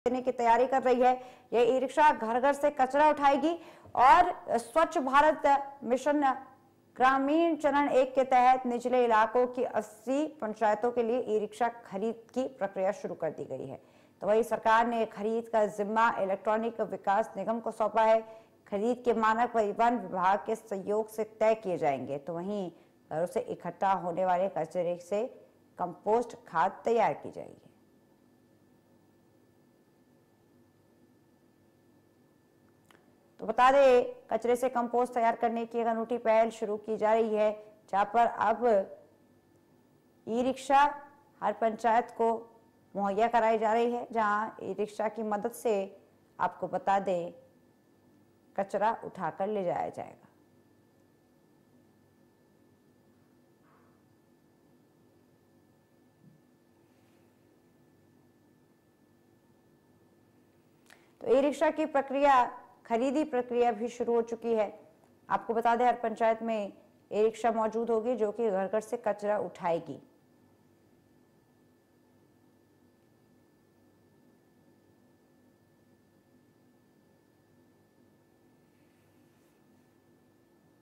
Breaking News: हिमाचल में ई-रिक्शा खरीद की प्रक्रिया शुरू | India News Haryana
हिमाचल की हर पंचायत को मिलेगा ई-रिक्शा। घर-घर से कचरा  उठाएगा ई-रिक्शा। कचरे से कंपोस्ट खाद की जाएगी तैयार। ई-रिक्शा खरीद की प्रक्रिया शुरू।